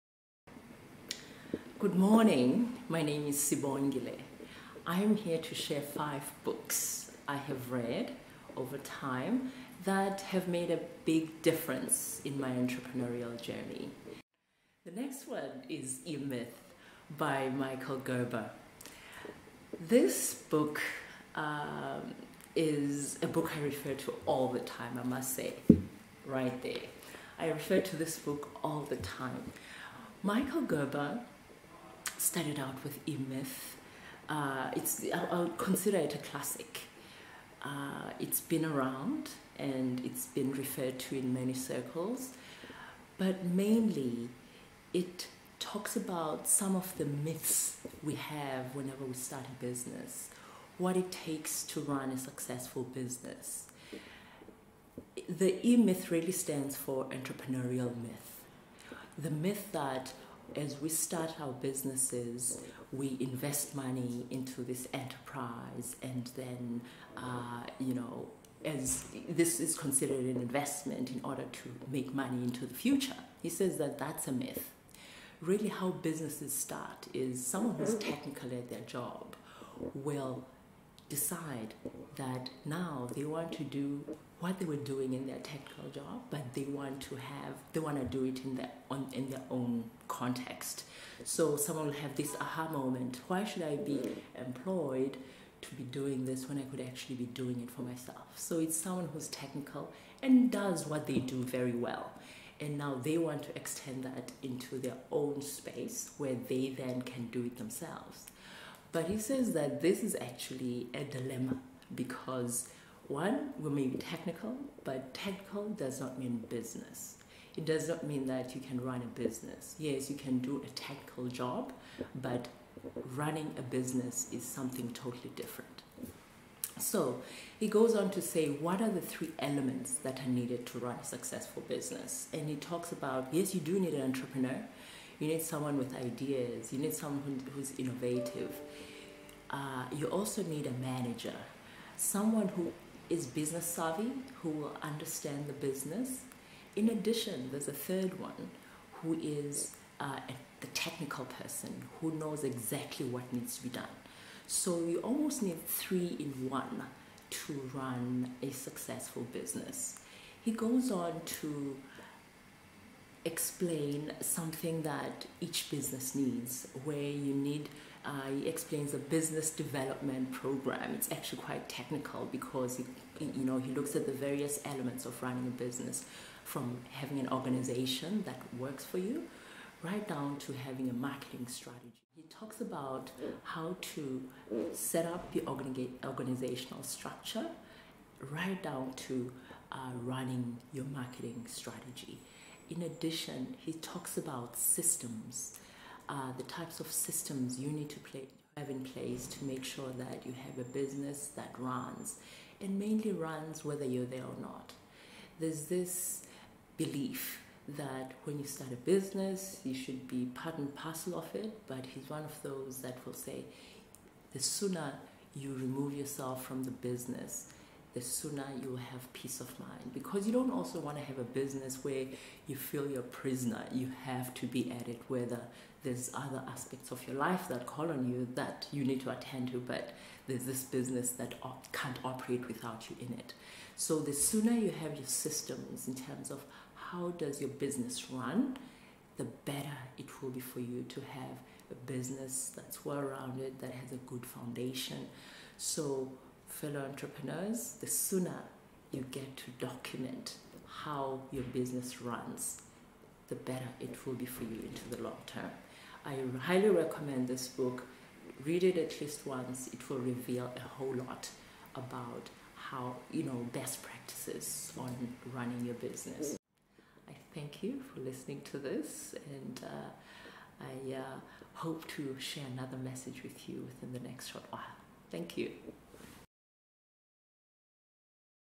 Book 2: ’The eMyth'' by Michael. E. Gerber
Sibongile Booi, founder of Second Office shares 5 books that she has read over time that have inspired and made a difference to her entrepreneurial journey.